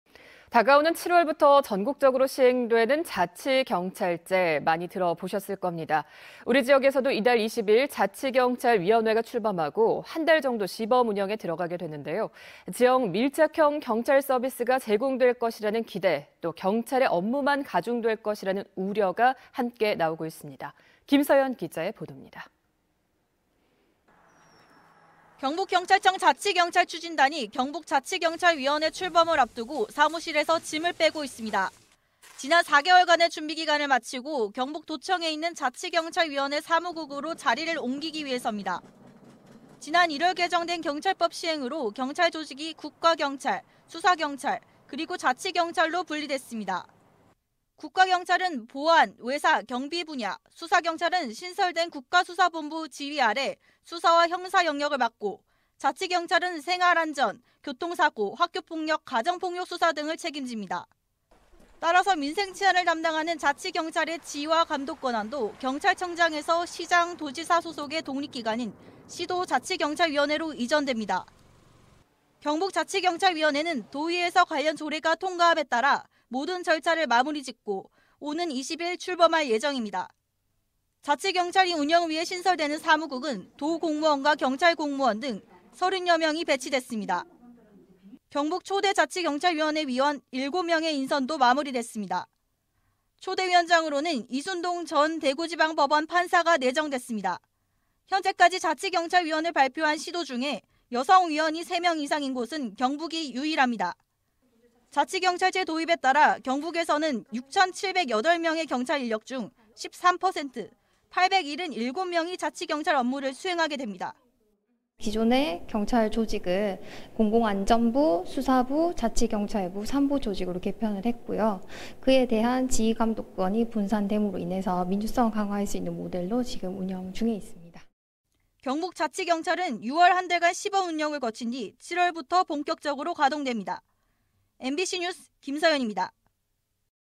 [대구MBC뉴스] 경북 자치경찰 20일 출범..시범운영 후 7월 시행
내용: 경북 자치경찰 20일 출범..시범운영 후 7월 시행 - 김서현 기자 (안동M)